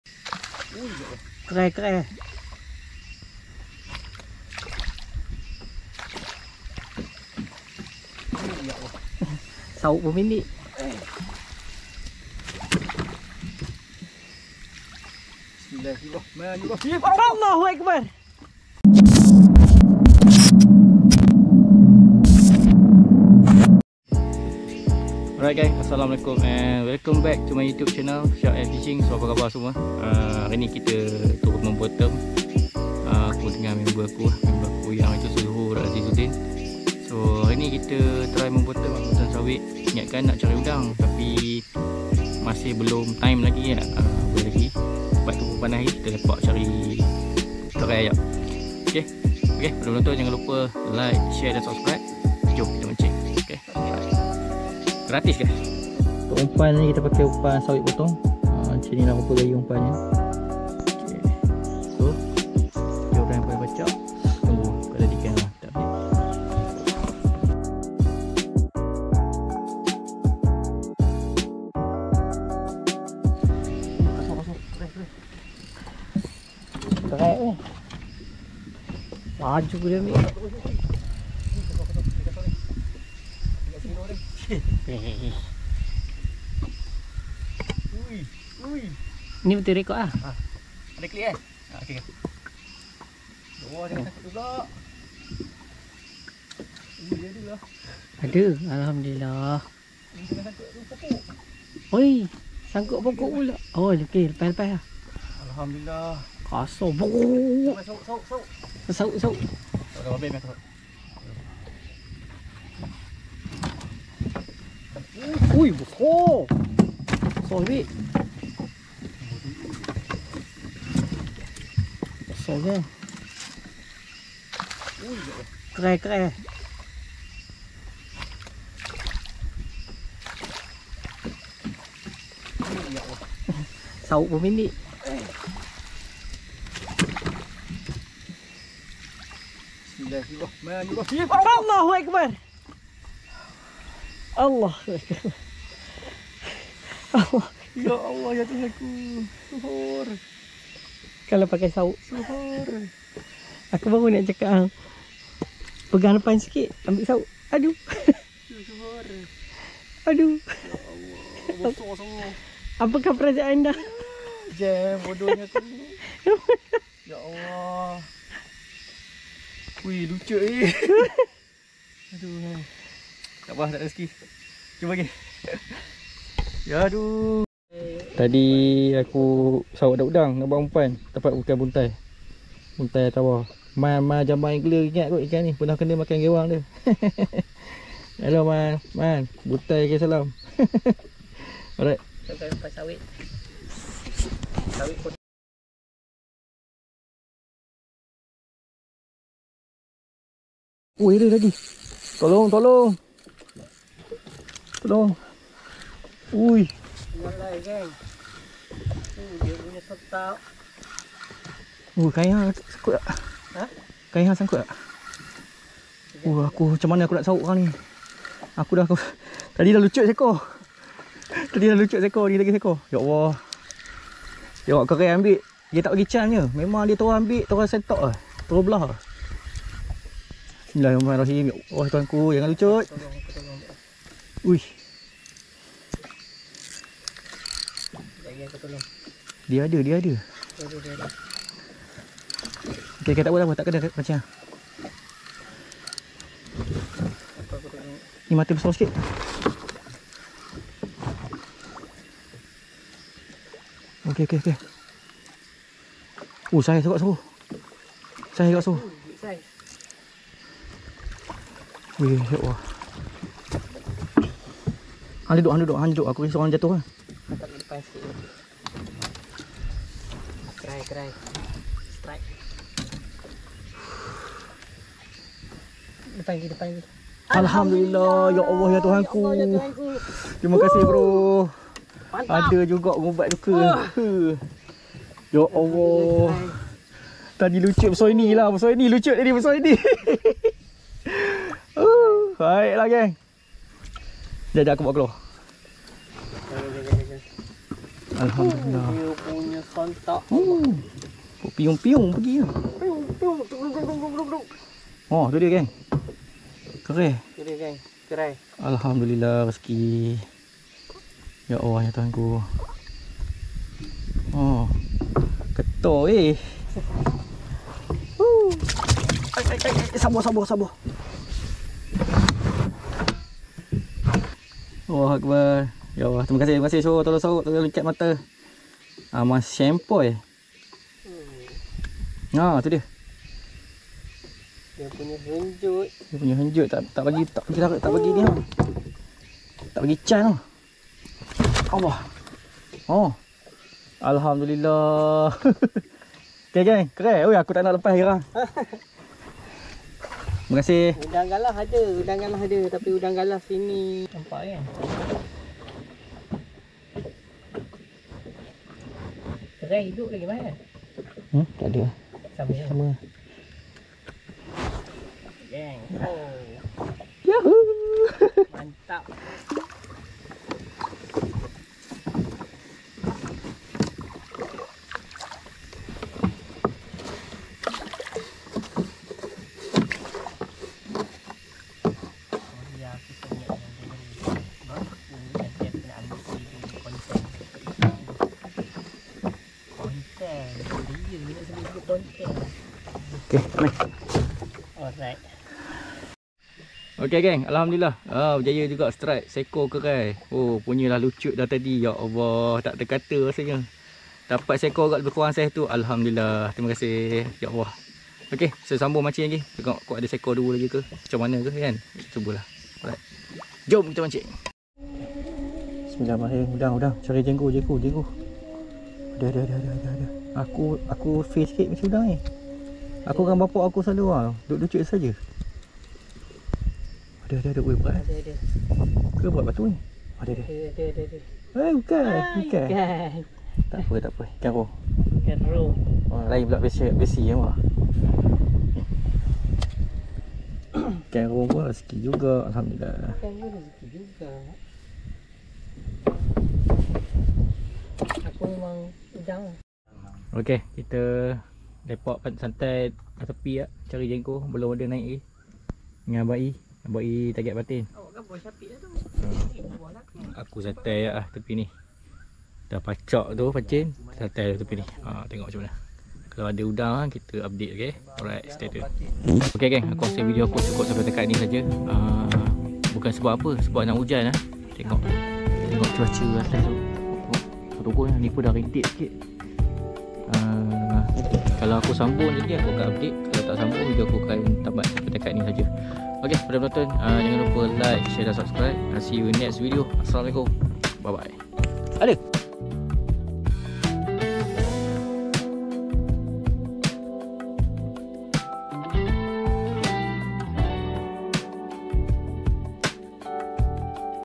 MANCING IKAN KERAI DI SUNGAI PERAK (IKAN INI LAGI SEDAP DARI IKAN SEBARAU,TENGALAN)
Trip santai memancing dinsekitar sungai perak bersama @ShuhurAzizuddinracun dari  @WAN_FISH_HUNTER